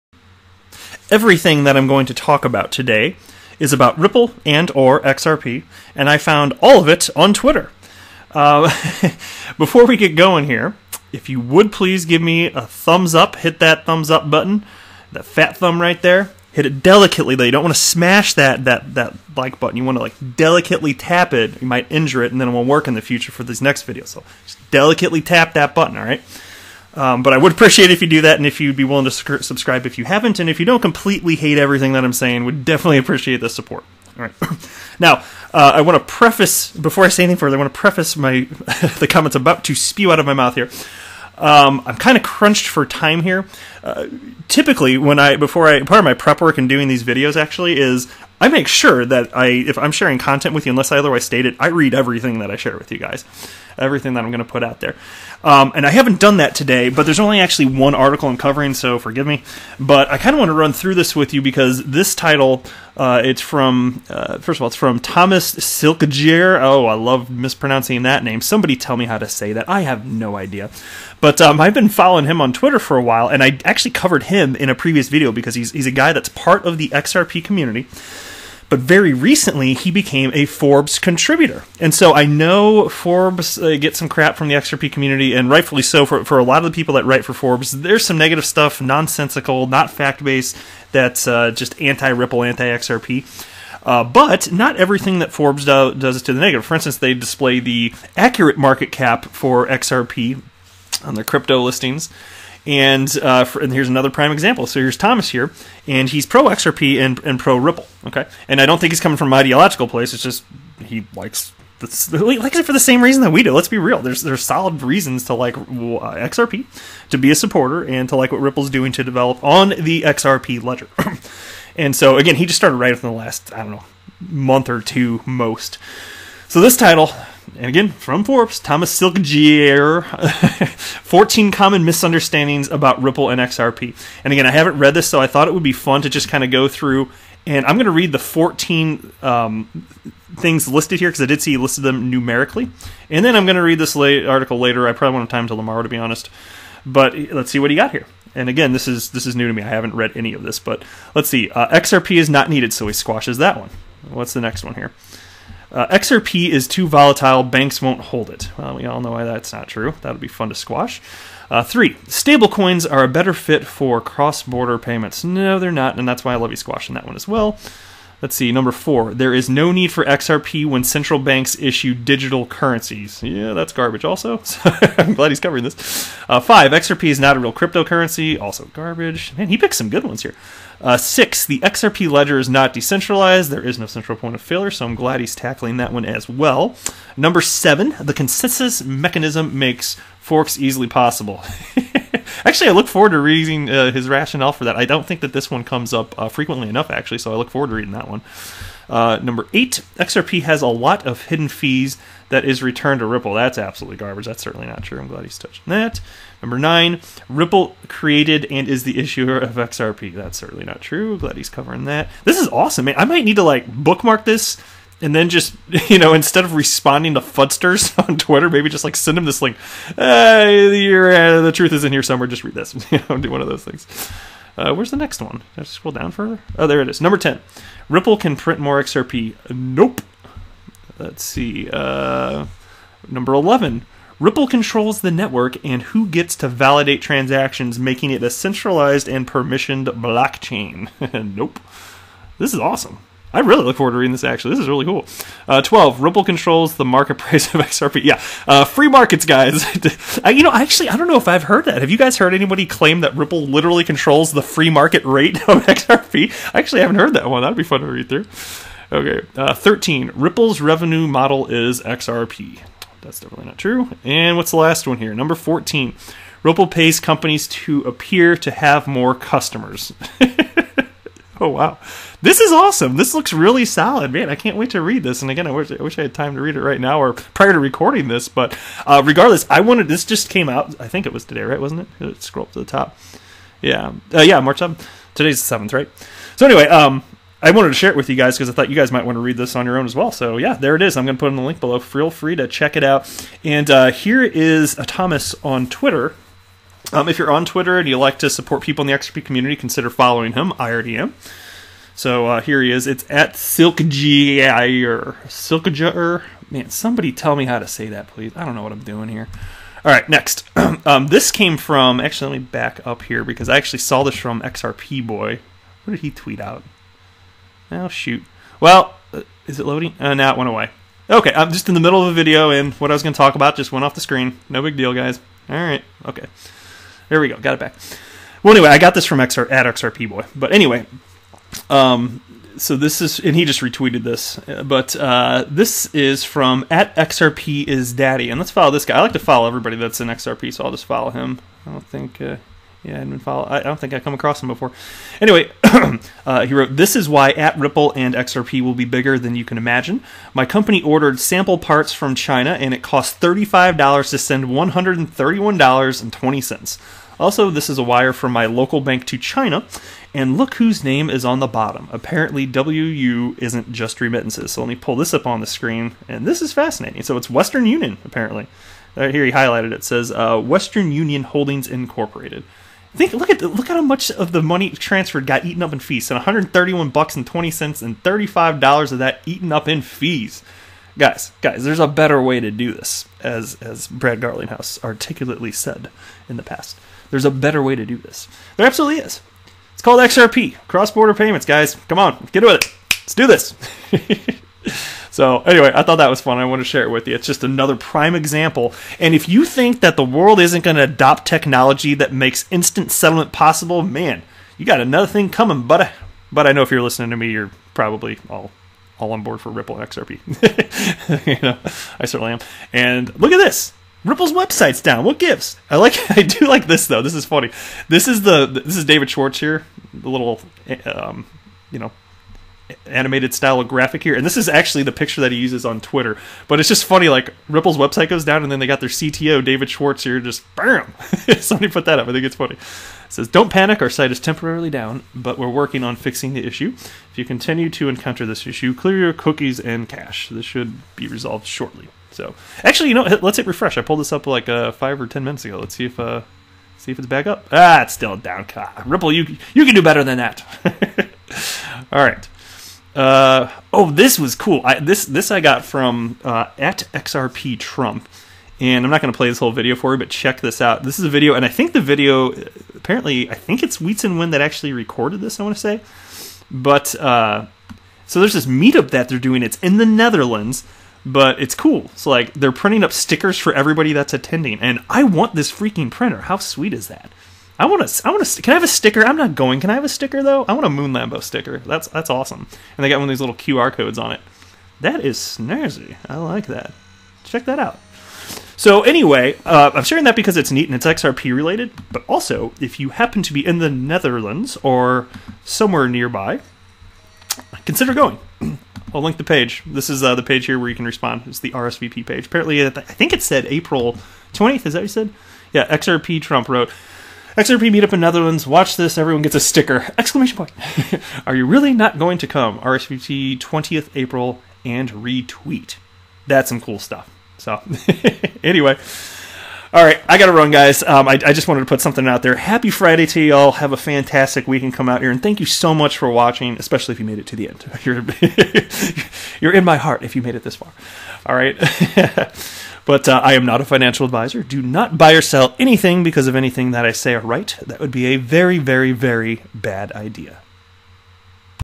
14 Common Misunderstandings About Ripple & XRP
Busting FUD like it ain't no thang.